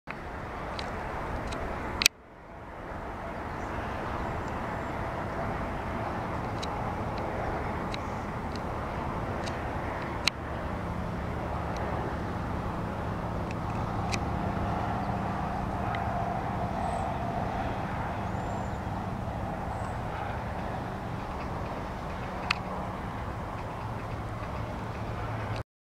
spider documentation ⊰shrooms⊱
finishing up wrapping its presumably liquified mummy and off he goes. fascination and bad camerawork thanks to my friends from the forest.